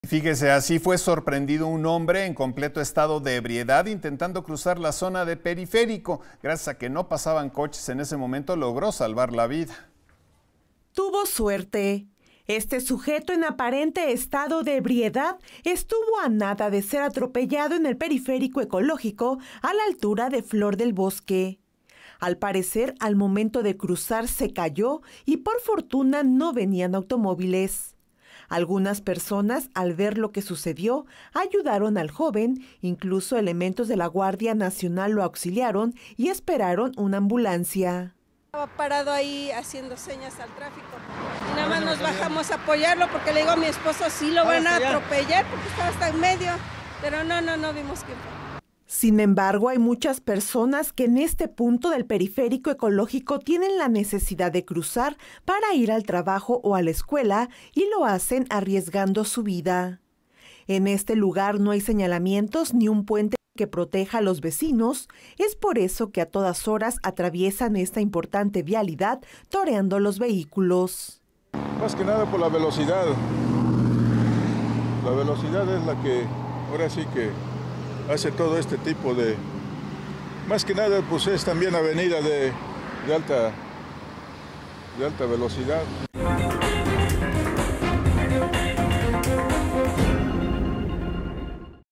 Estaba hasta las manitas, tuvo suerte
Así fue sorprendido un hombre en completo estado de ebriedad al cruzar el Periférico Ecológico , tuvo suerte de no pasar a un accidente mayor